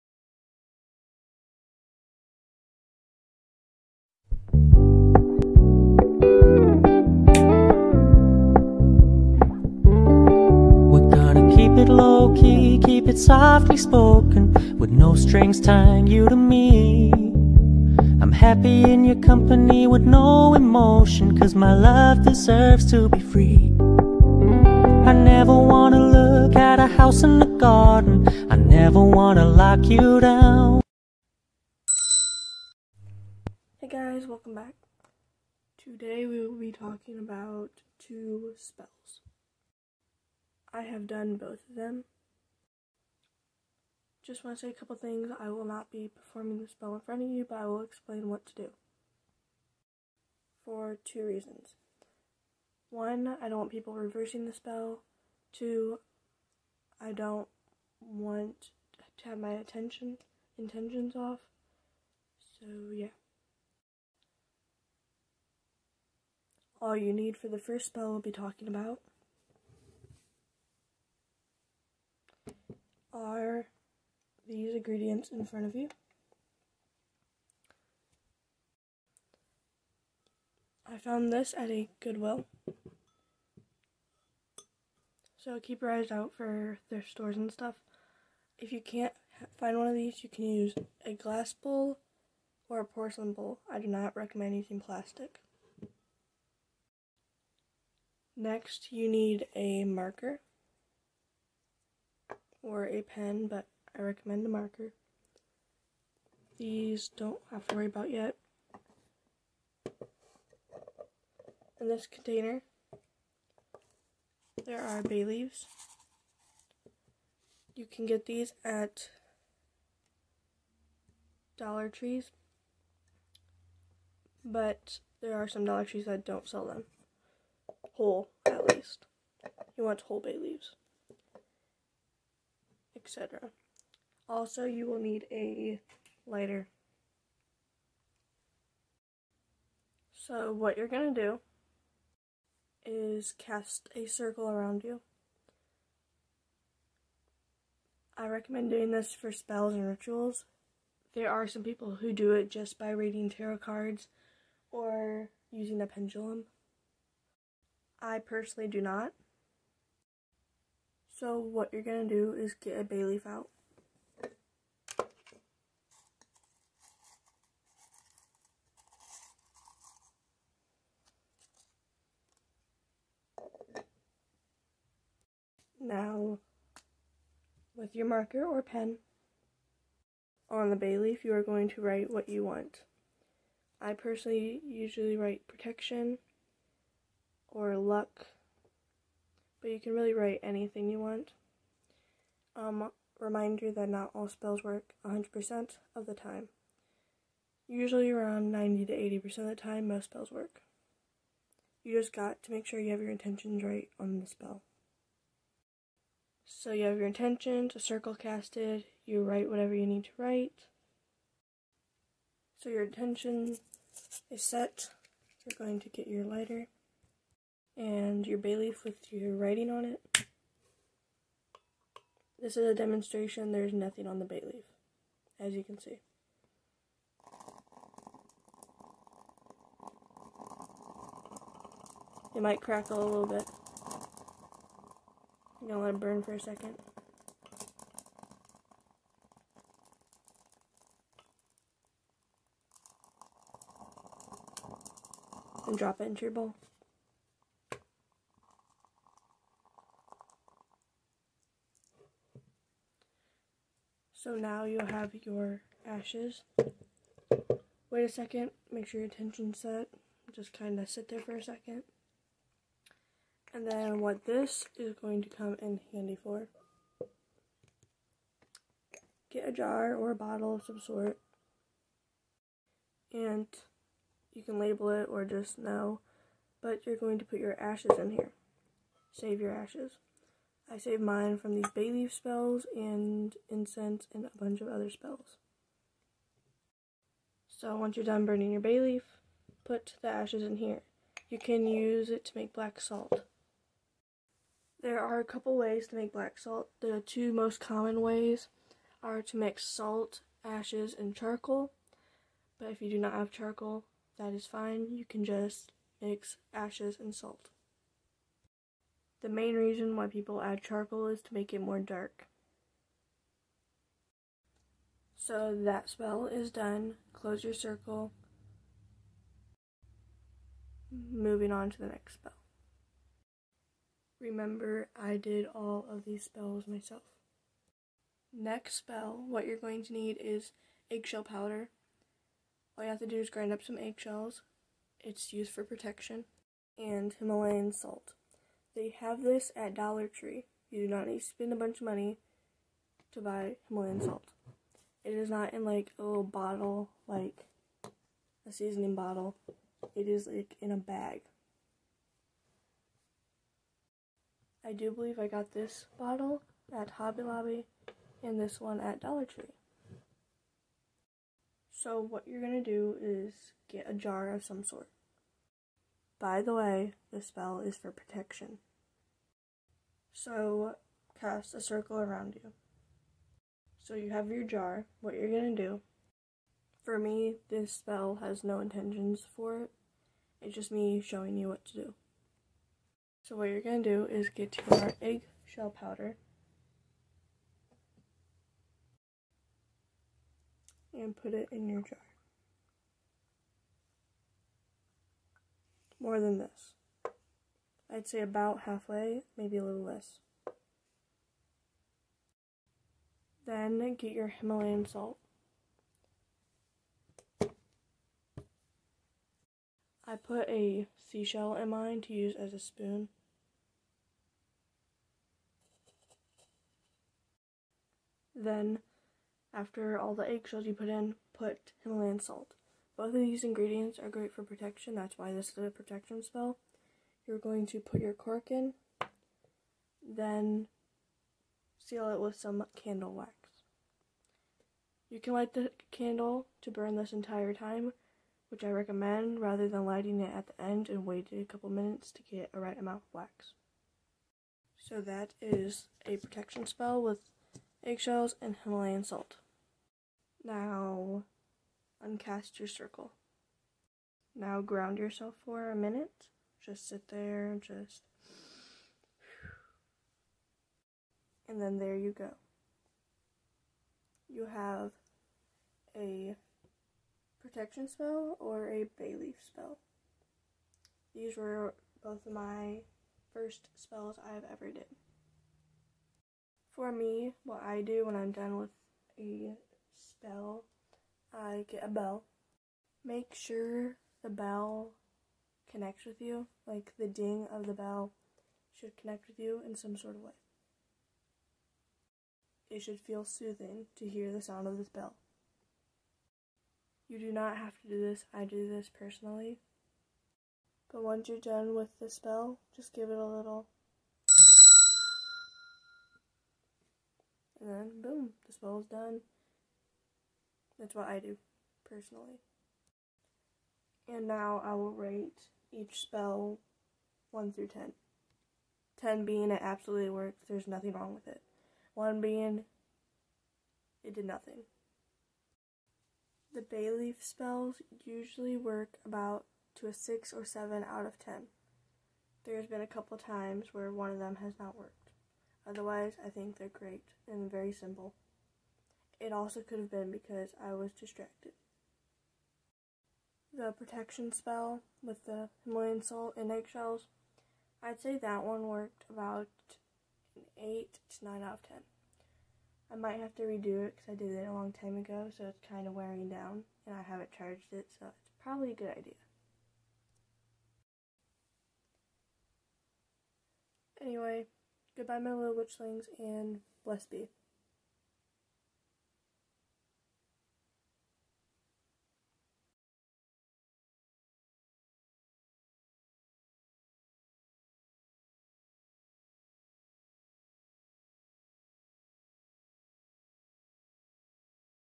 protection spell and Bailey spell, simple
baby witches if you have been practicing for a long time now, like over a year or two .. and you want to do a spell I recommend these... just be careful and do your research!!